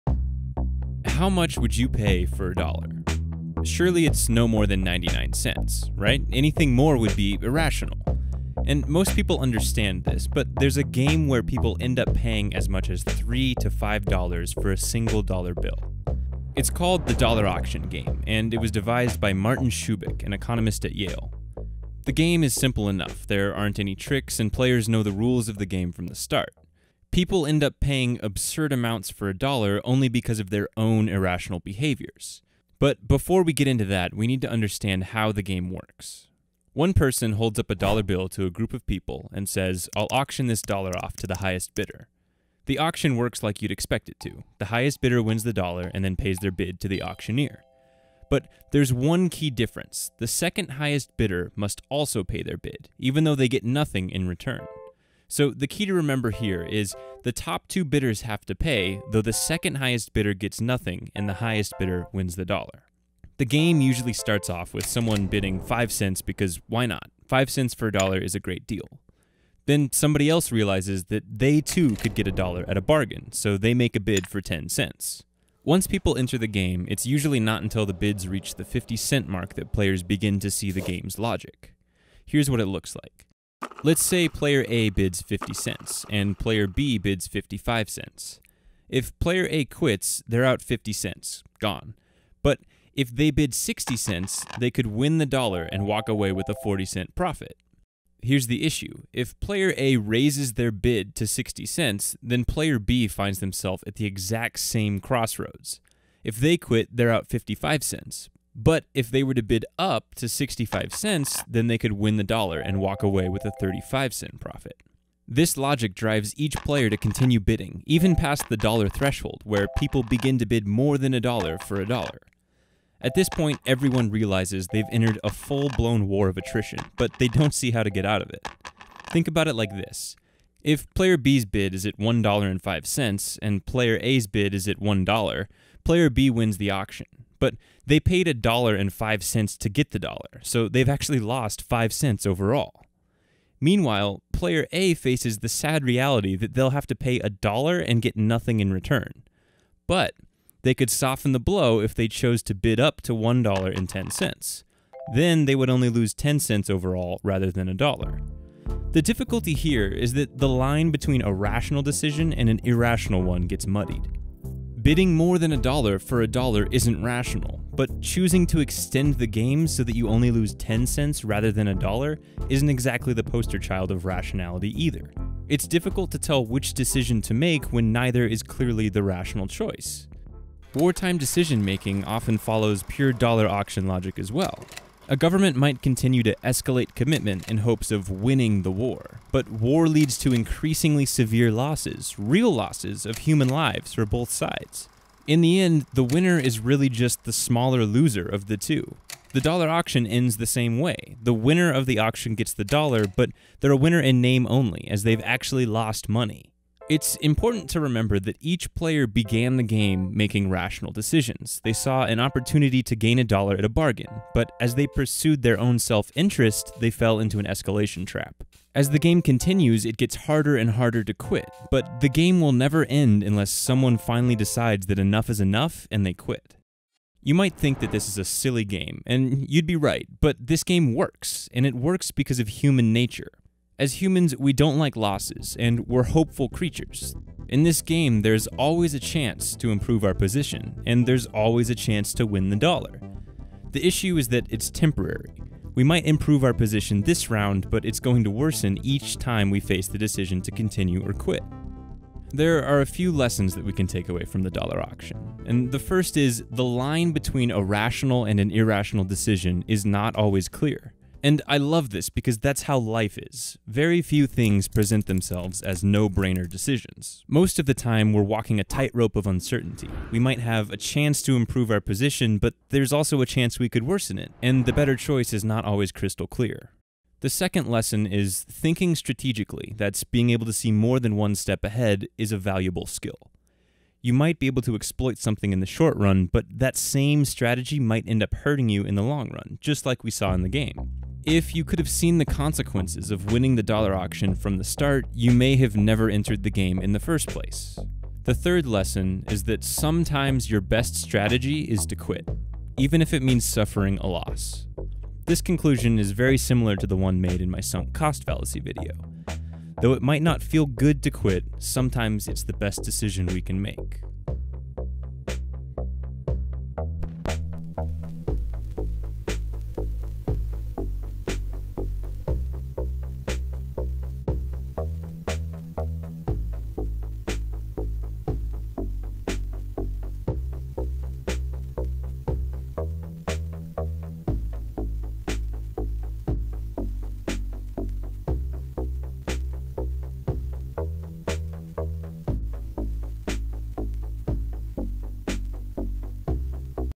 The (Ir)rationality of the Dollar Auction
The Dollar Auction is a simple game devised by economist Martin Shubik in the 1970s.

The beauty of the game is it's remarkable ability to illustrate the frequent irrational behaviors that we humans engage in.

The Dollar Auction game shows how the rational pursuit of our own self interest can lead us into an escalation trap and make us worse off.

Though the game may appear to be silly, we engage in all sorts of behaviors in which we irrationally escalate our commitment in hopes of something better.

Video + Sound + Script by Alex Belser.